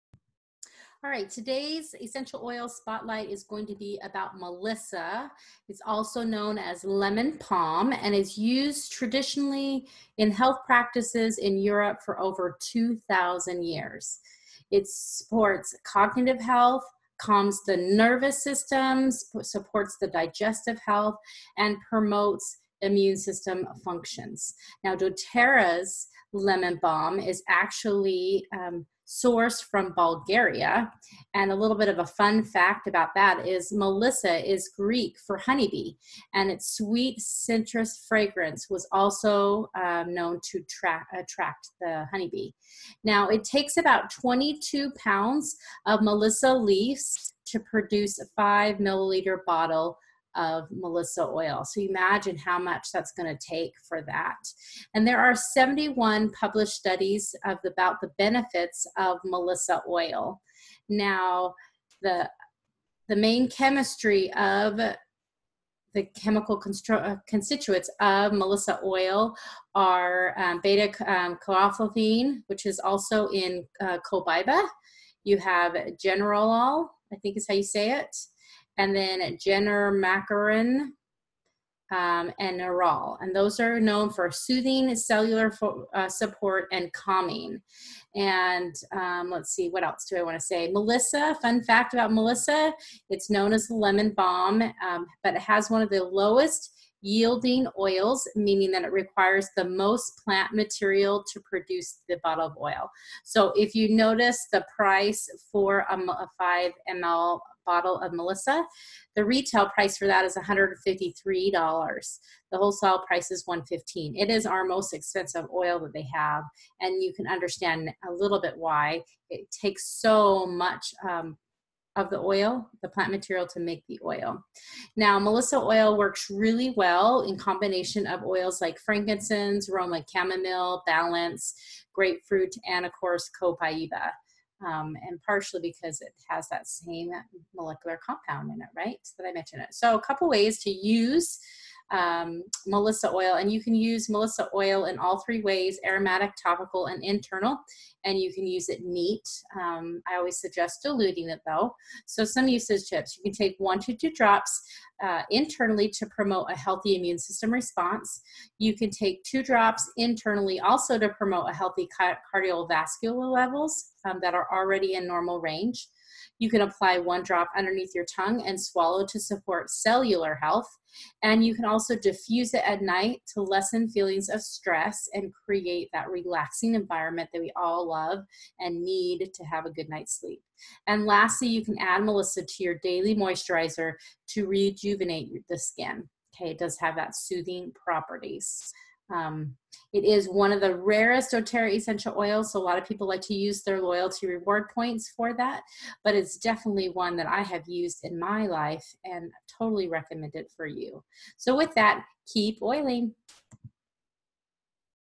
Melissa Spotlight
Today we are highlighting the benefits of Melissa. Did you know you can diffuse this oil at night or rub on your forehead, shoulders, or chest to help lessen feelings of stress and promote emotional well-being? This small, but mighty essential calms tension and nerves. Learn more now.